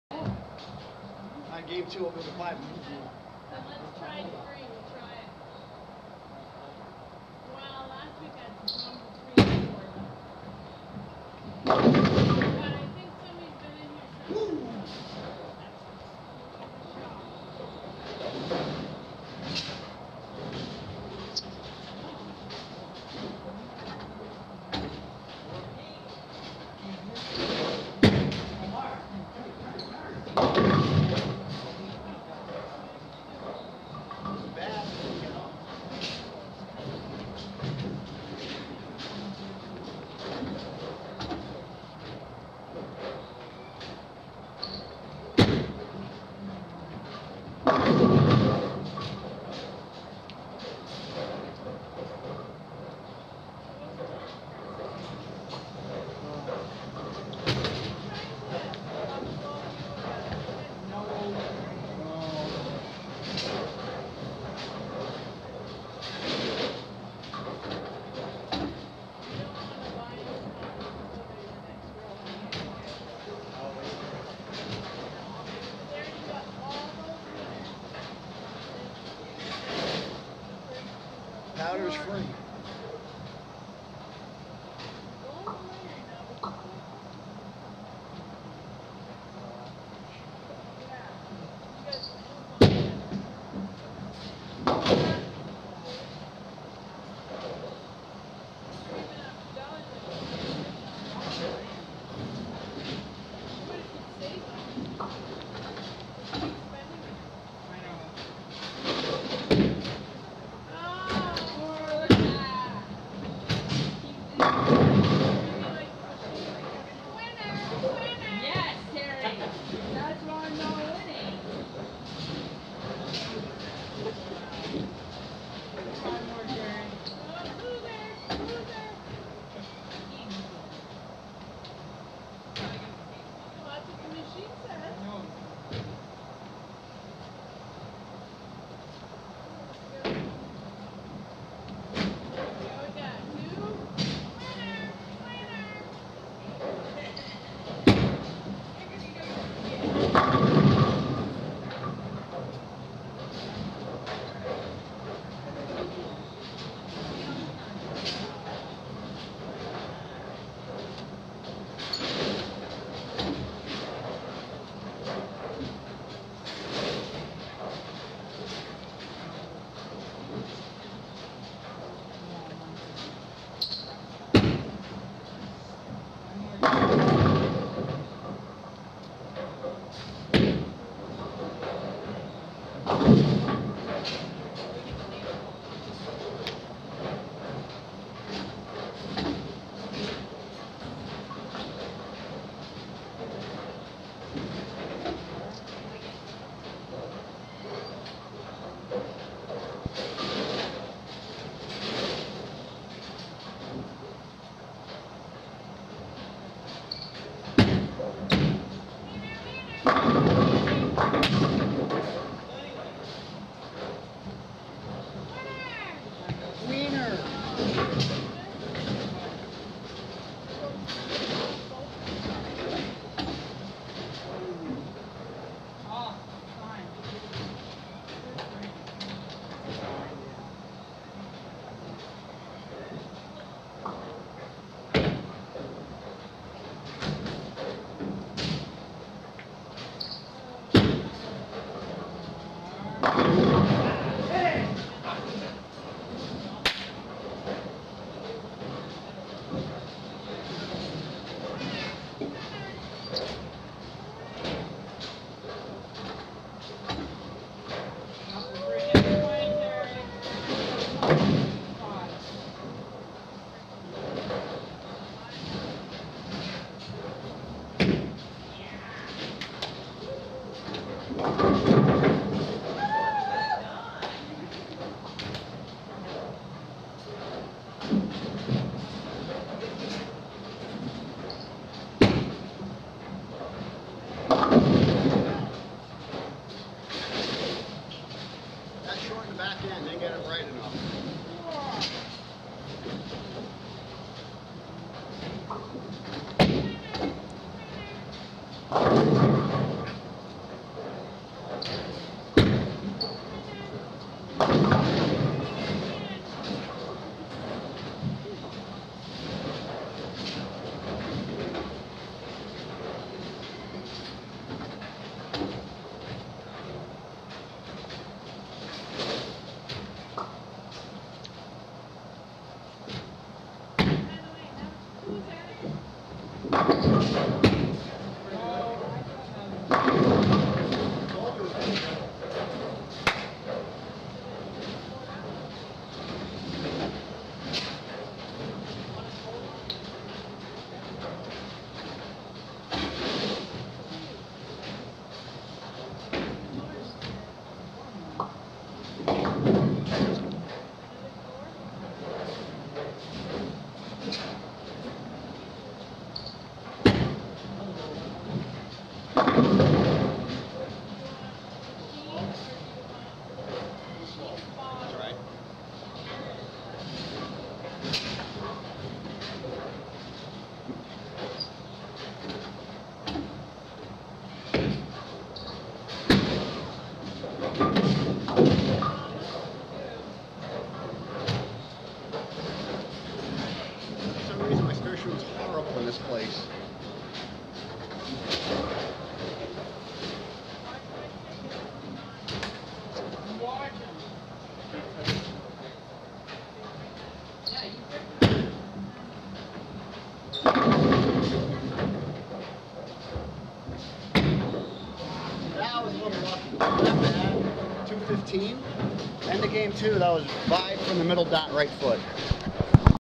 Fall BARN Stop 1, Dairy Center; game 2 of 5
The Dairy Center house shot is a more oil than normal house shot. Send it wide, it can sail, pull it and it will go thru the nose. Playing further left this game and swinging it a bit with a Hammer Vibe.